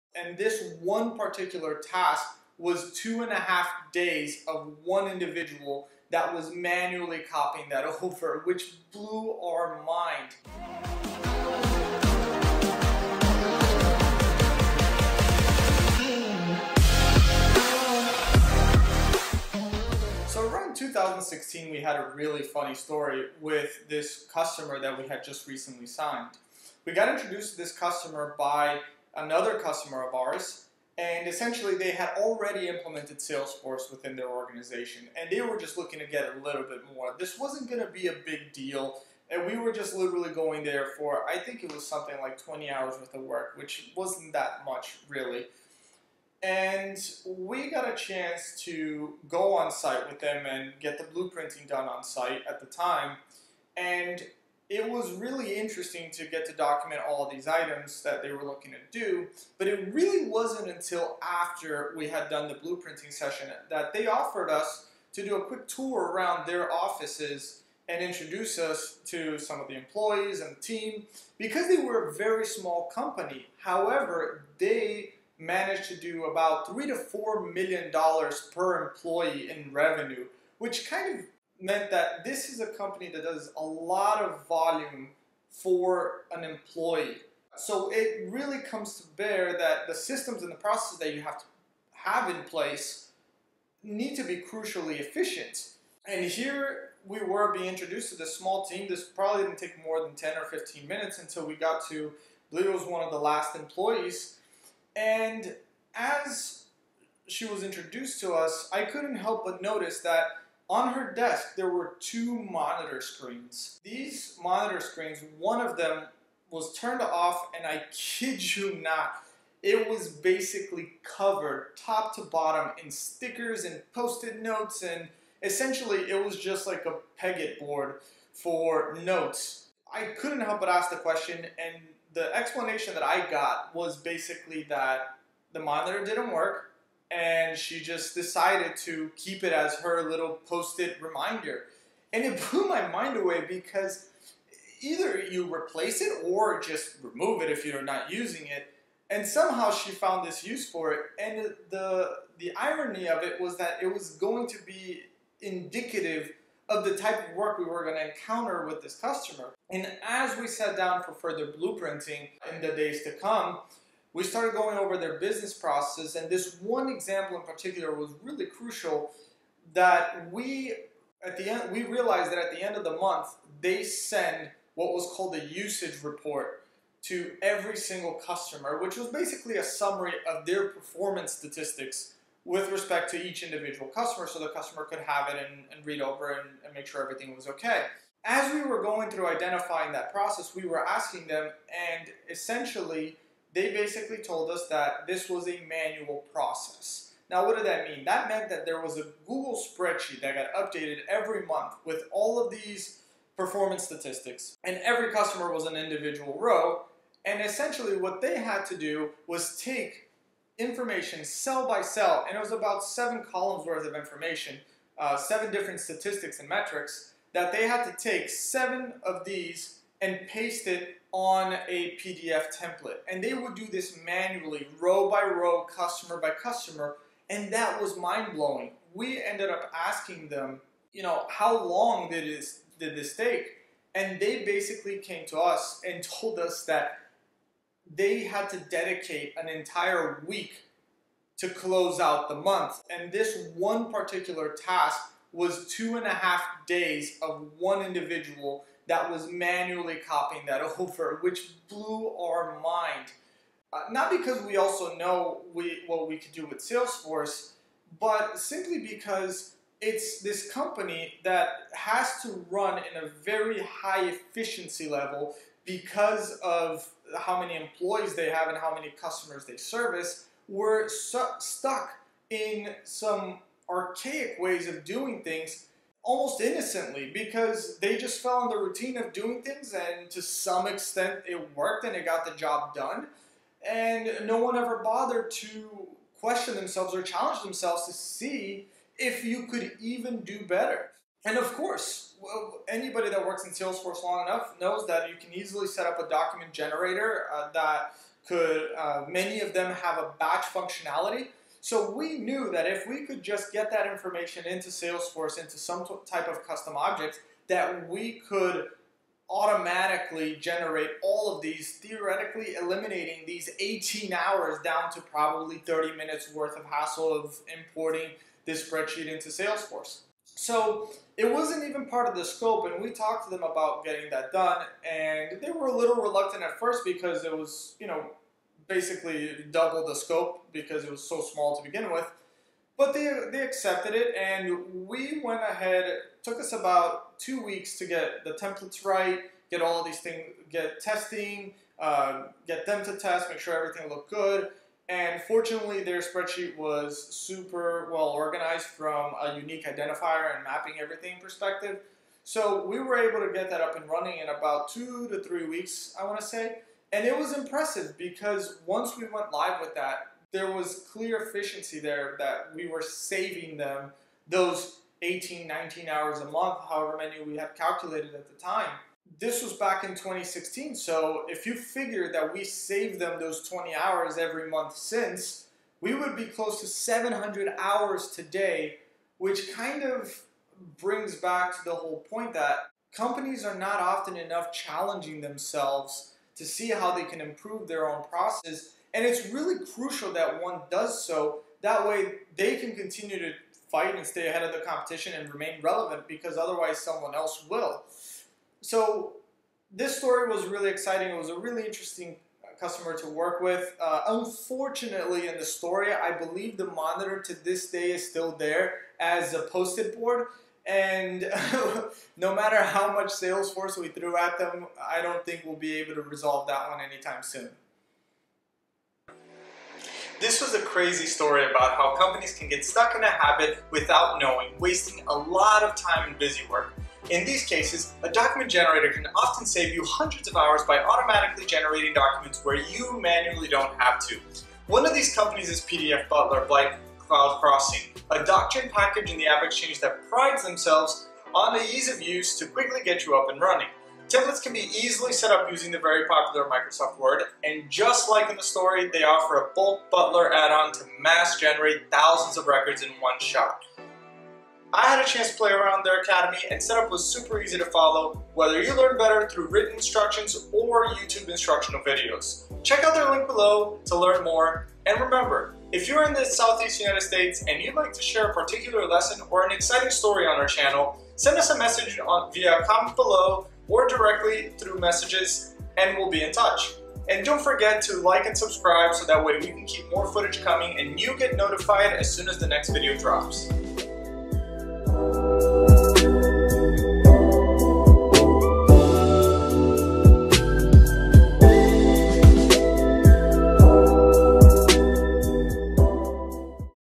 Implement Automated Processes & Save Time in Salesforce w/ a Document Generator from the AppExchange
Getting in a rhythm doesn't necessarily mean you're an efficient organization. Sometimes we get too stuck in a way of doing things that often one forgets to ask "is there a better way to accomplish what we're doing here?"

For one company, I got to experience an epic example that includes monitors, post-it notes, and half a week's worth of copy/pasting.

Looking to share your story? Fill out the form and tell us more

*** CHAPTERS ***

0:00 Intro
0:48 Case study blueprint & Company info
1:45 "The Monitors"
2:44 Usage Report (Manual Processes)
3:50 How much time was being wasted
4:45 Introducing a solution to save time
5:58 How much time was saved
6:57 PDF Butler